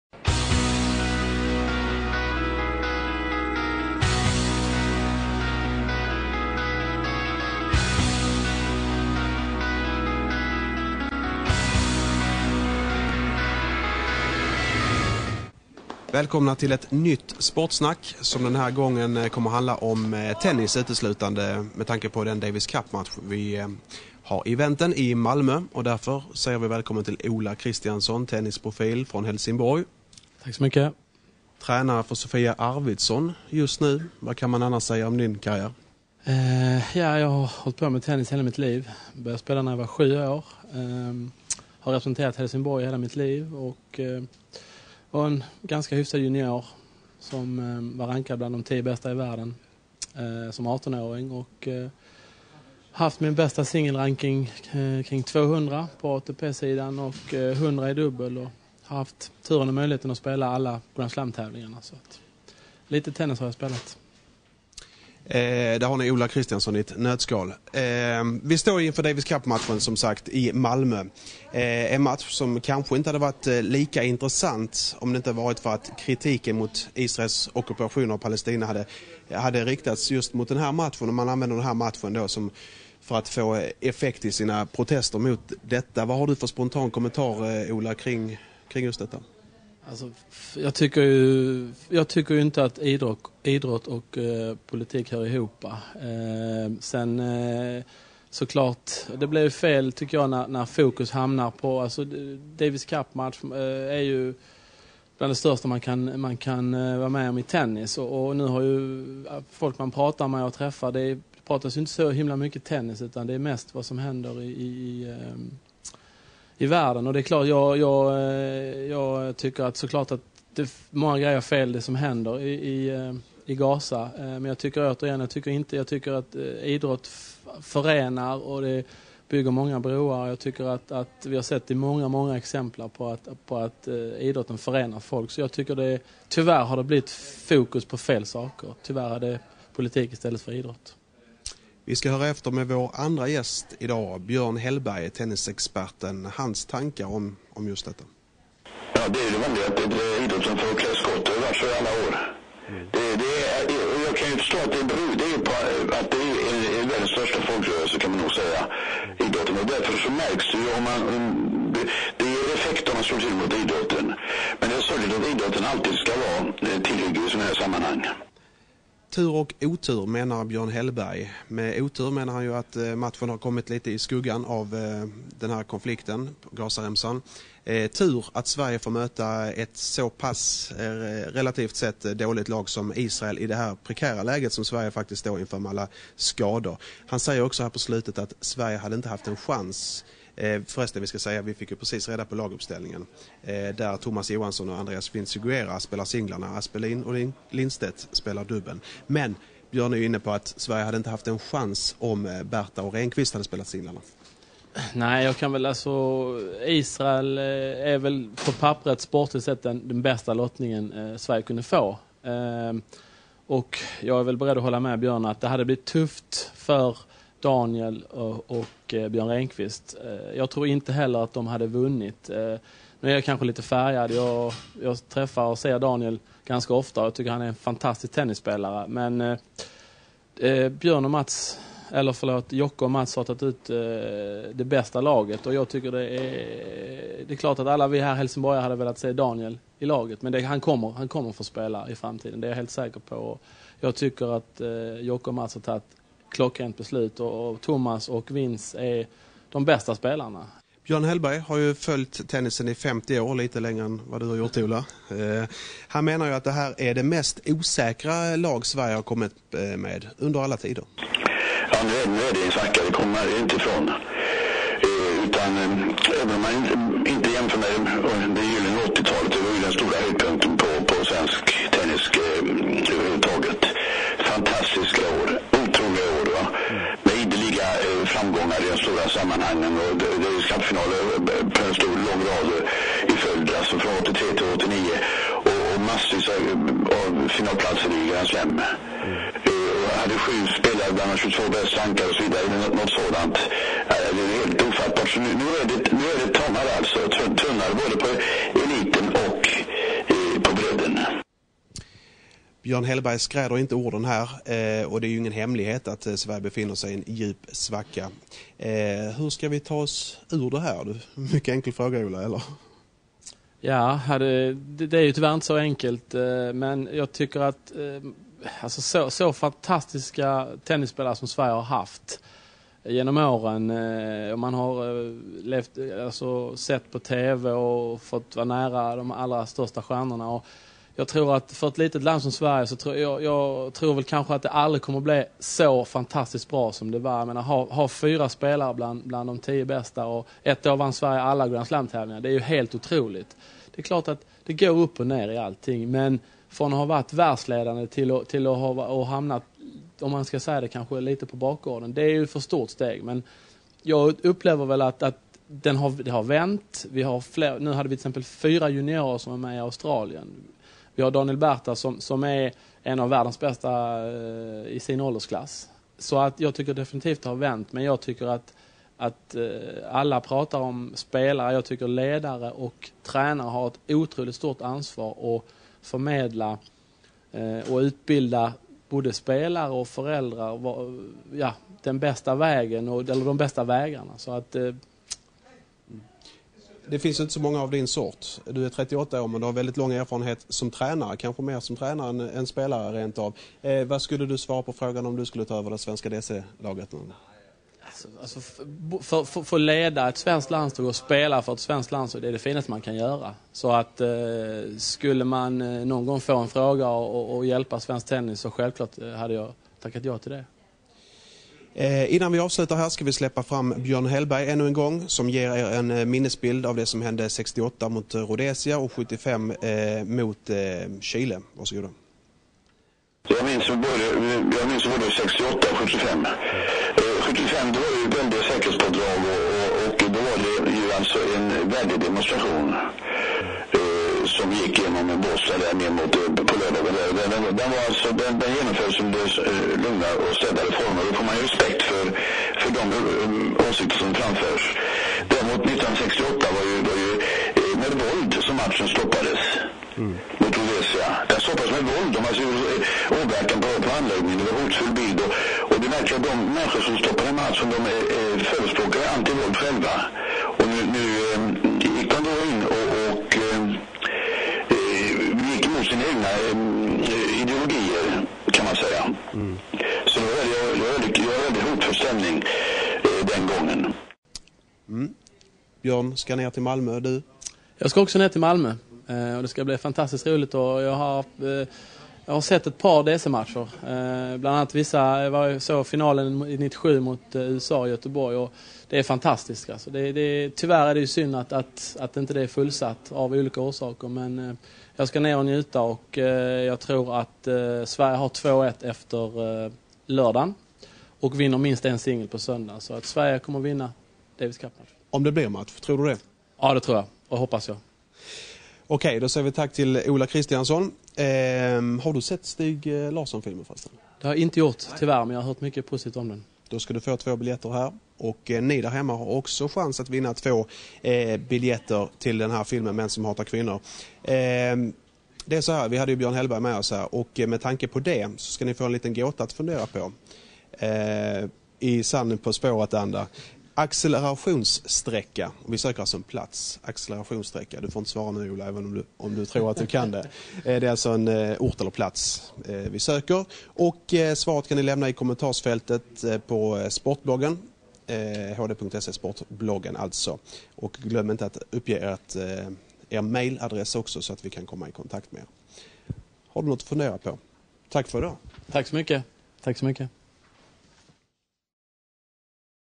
Sportsnack om Davis Cup
I veckans Sportsnack diskuterar vi den stundande Davis Cup-matchen mellan Sverige och Israel. Men vi pratar också Sveriges tennisframtid och så bjuder vi på en gåta i sann Hellberg-anda. Gäst i studion är tennisprofilen Ola Kristiansson och på en sprakande telefonlinje... just det - Björn Hellberg.

Publicerad på hd.se torsdag 5 mars 2009.